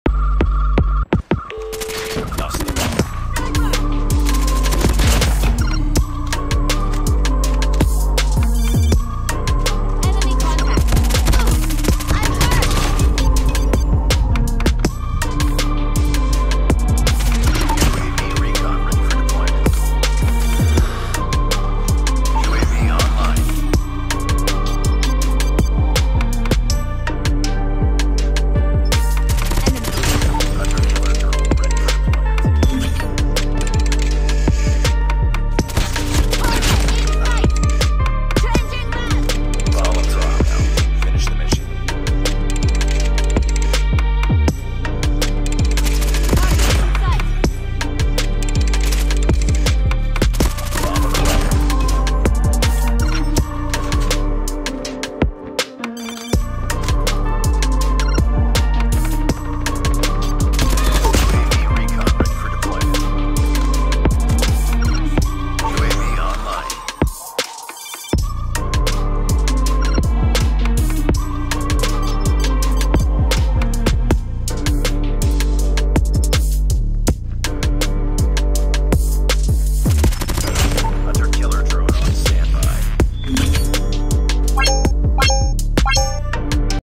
INSANE 1V5 CLUTCH(s) : AS VAL destroying lobbies 🤌🏼🧠
I tried this new build for AS VAL. It is on FIREEEEE 🔥
WATCH ME ACE WATCH ME NAE NAE